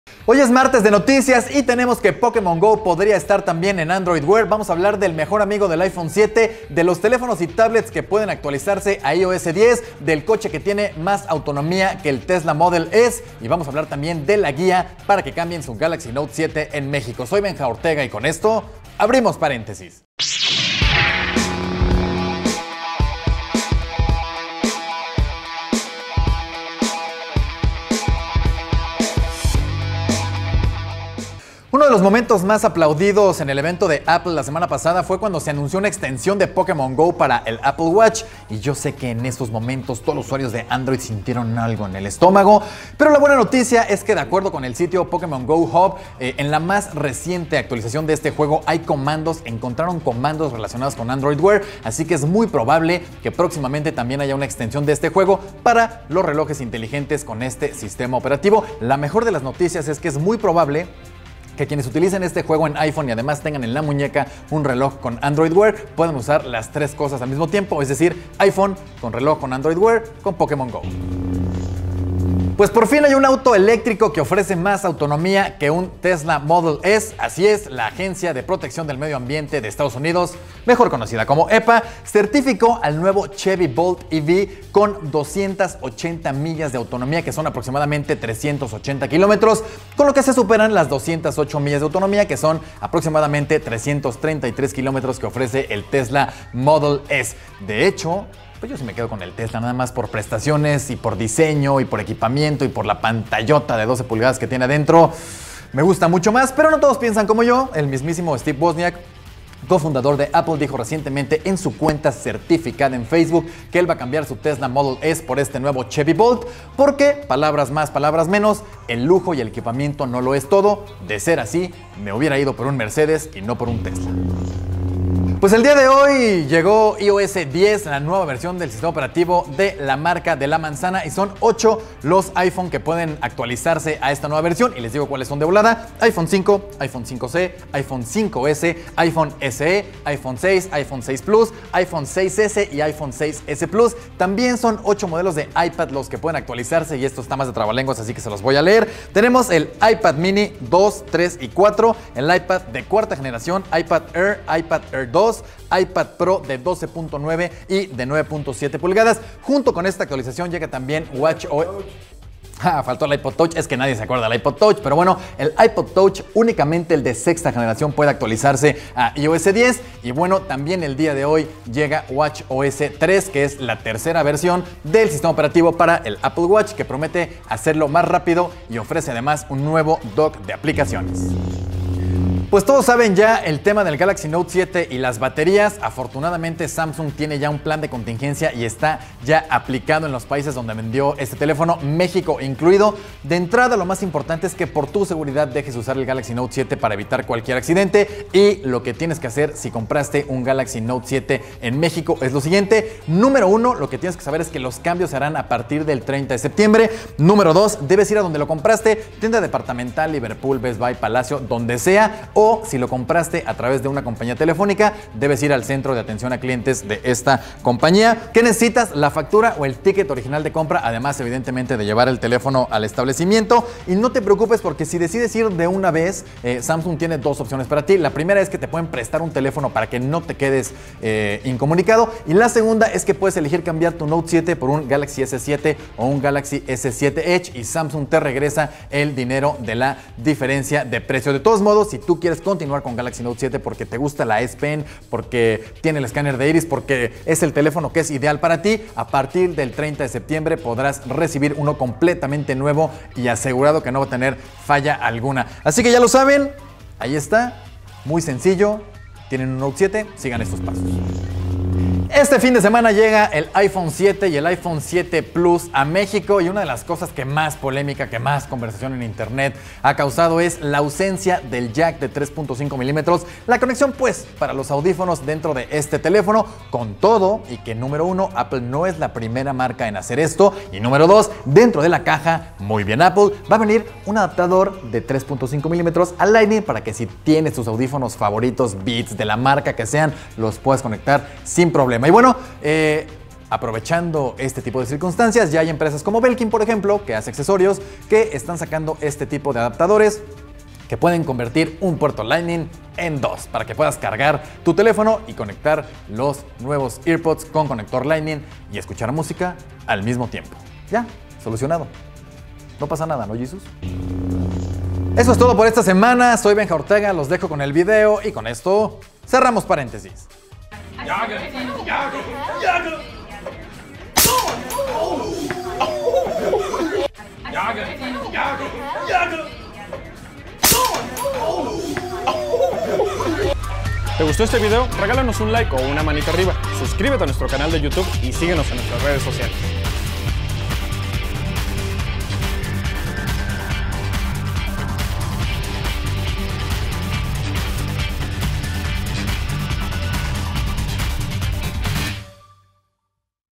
MartesDeNoticias: Guía para cambiar tu Galaxy Note 7
Hoy es MartesDeNoticias y tenemos los gadgets Apple que actualizan a iOS10, el accesorio que promete ser el mejor amigo de los iPhone 7, Pokémon Go en Android Wear, el auto eléctrico que le gana en autonomía al Tesla Model S y la Guía para cambiar tu Galaxy Note 7 en México. ¡Dale play!